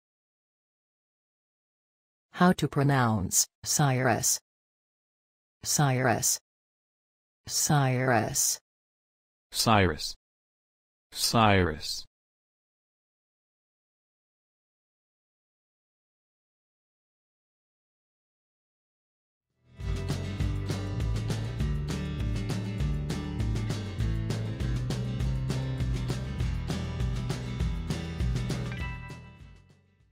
Cyrus pronunciation
How to pronounce Cyrus in English? Listen with us.
What is the correct pronunciation of the word Cyrus in everyday English? Speak as the Americans.
How to write the word Cyrus? Watch and learn.

Learn how to pronounce Cyrus.